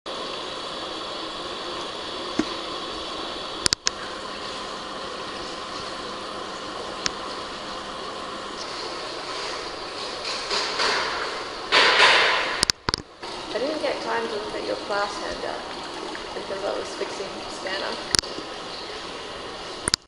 WaihoModelRunning
Short video of 1:3,000 scale model of the Waiho River